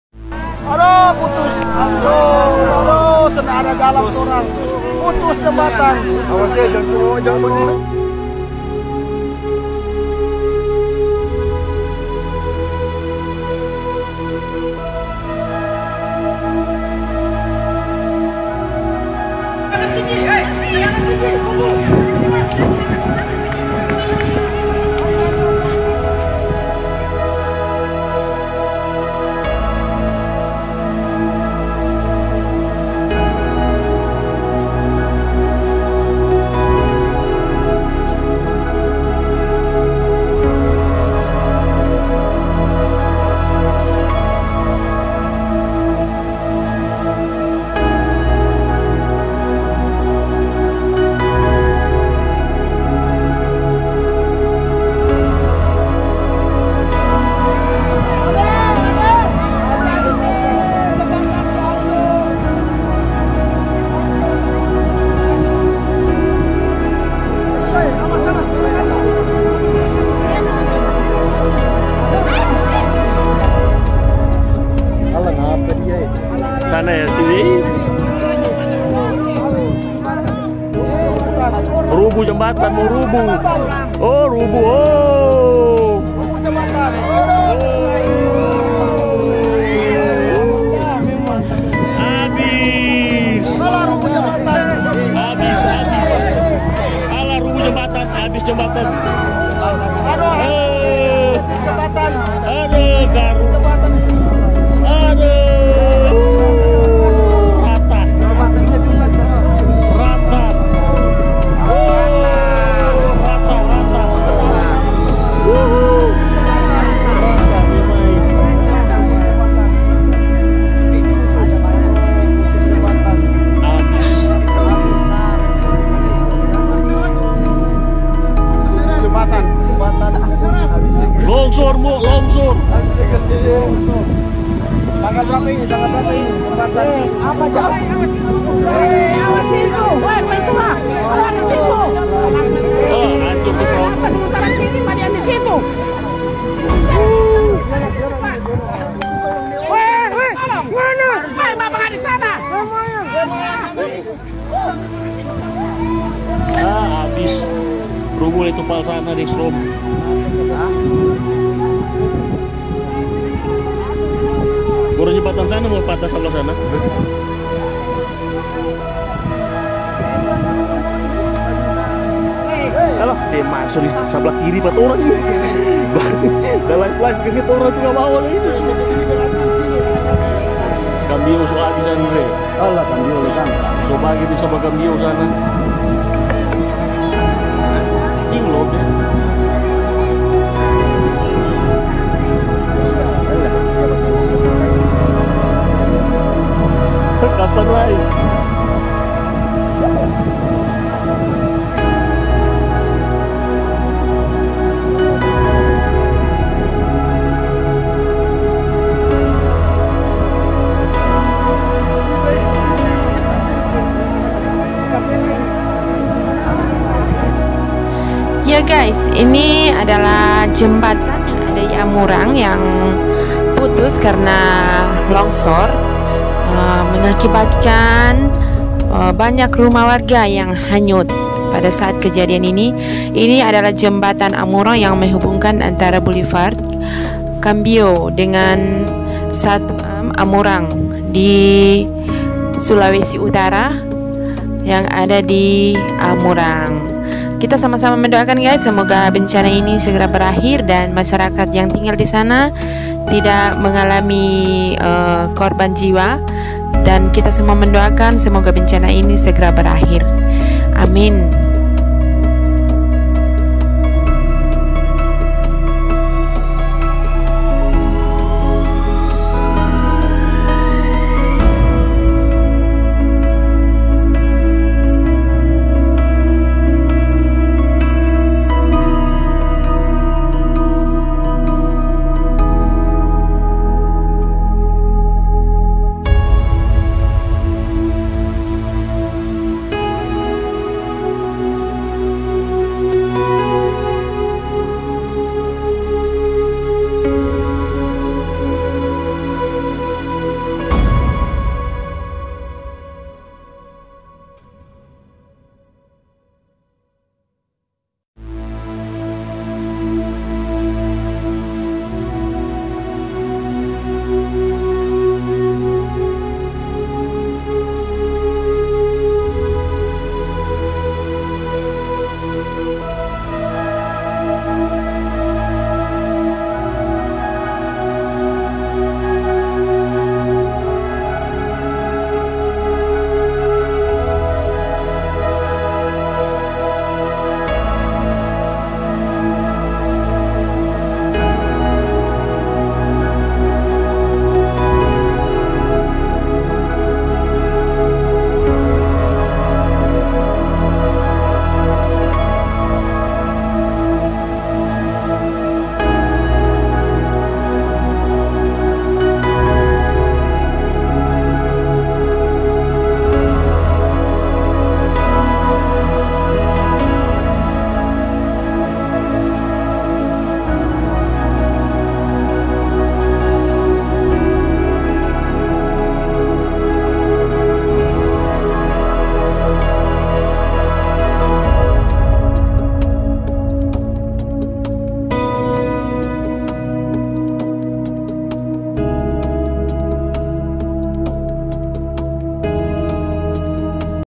Jembatan ambruk..⁉️||PRAY FOR AMURANG||Sulawesi Utara
Telah terjadi hal yang kita tidak inginkan bencana alam terjadi di Amurang mengakibatkan jembatan ambruk dan membuat rumah masyarakat banyak yang hanyut,jembatan ini adalah jembatan yang menghubungkan antara Boulevar kambiow.
semoga bencana ini tidak berkepanjangan dan mengakibatkan korban jiwa,kita sama sama mendoakan bencana ini segera berakhir.Amin